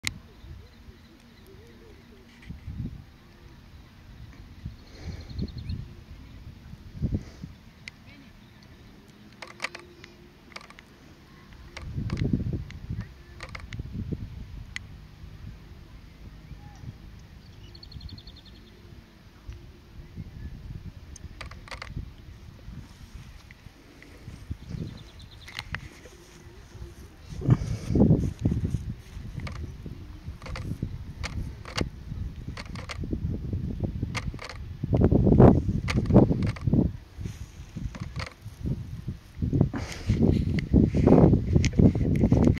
Ground Squirrel in the grass [4k]
In Summer 2019 I took several pictures of ground squirrels and this video very short. I really need to concentrate on filming a bit more the next times when I am on a photgraphy trip.